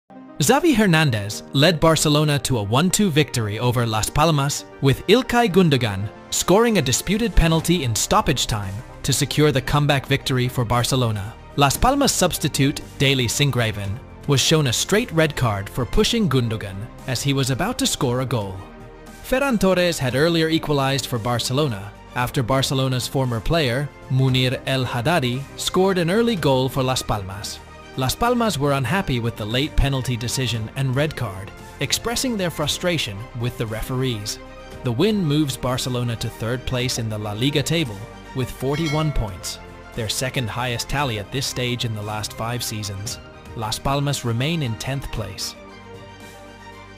UD Las Palmas vs Barcelona 1-2| Highlights| LaLiga
Xavi hernandez led Barcelona FC to a 1-2 victory over Las Palmas with Ilkay Gundogan scoring a disputed penalty in stoppage time to secure the comeback victory for Barcelona.
Las Palmas substitute Daley Sinkgraven was shown a straight red card for pushing Gundogan as he was about to score a goal.
Ferran Torres had earlier equalized for Barcelona after Barcelona's former player, Munir El Haddadi scored an early goal for Las Palmas.
Ud Las Palmas were unhappy with the late penalty decision and red card, expressing their frustration with the referees. ud Las palmas vs barca ended with drama. barcelona vs las palmas.